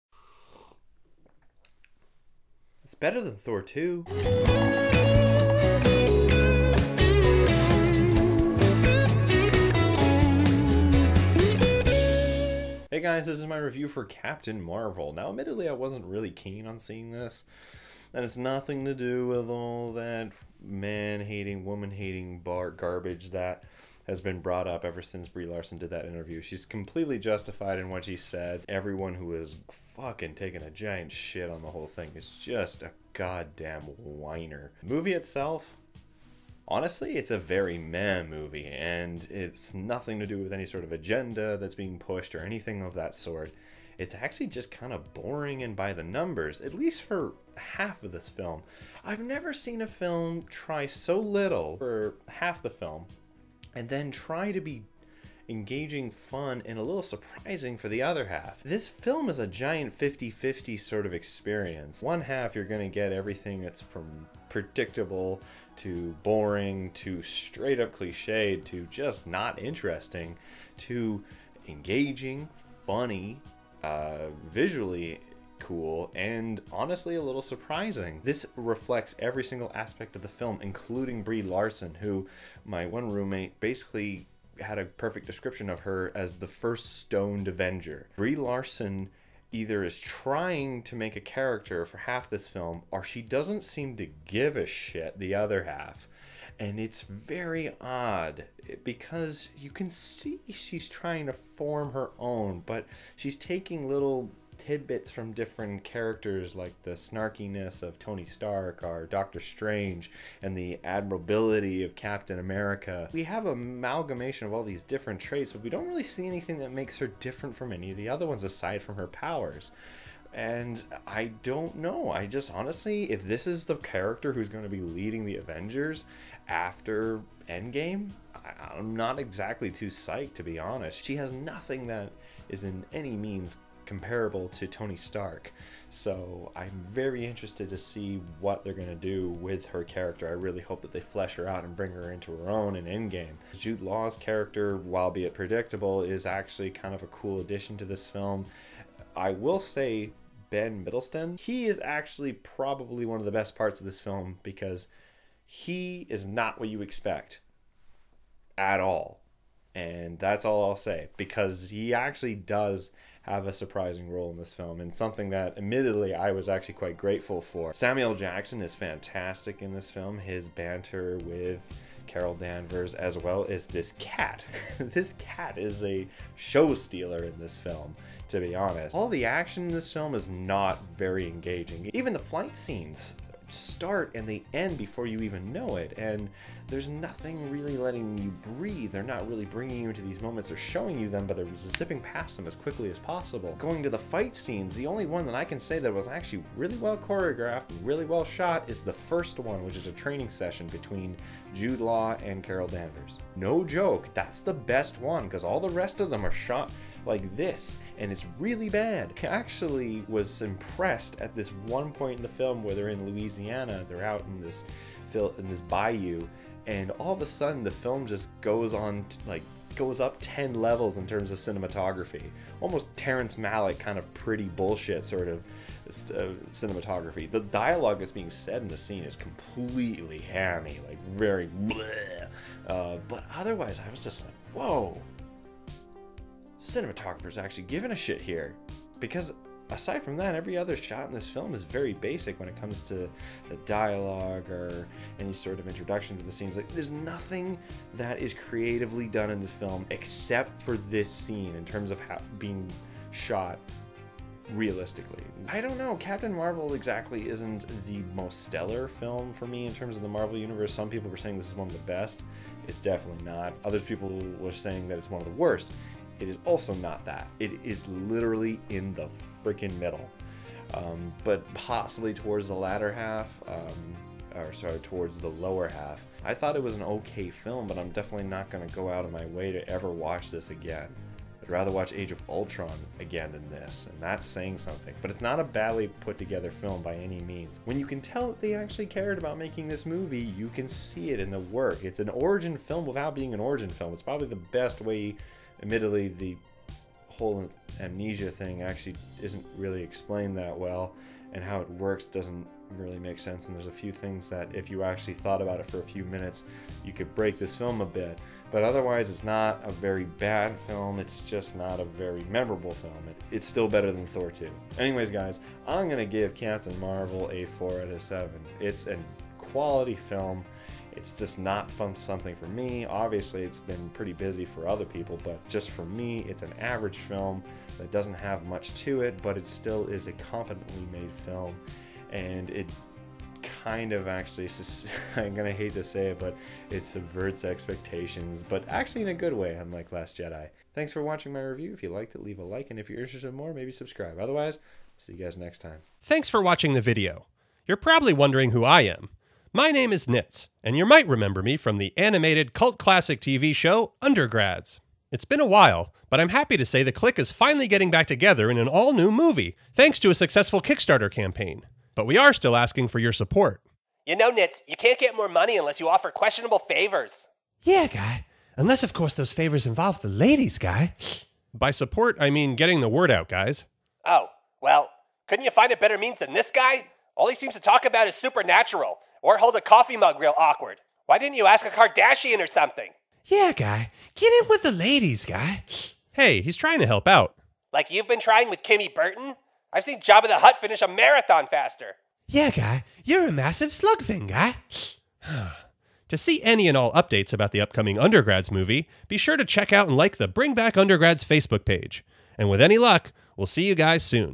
Captain Marvel Movie Review
So I finally saw Captain Marvel, mainly because I need to see it for Endgame. Oddly enough, this film is an amazing display of half not giving a crap, half trying to be surprising!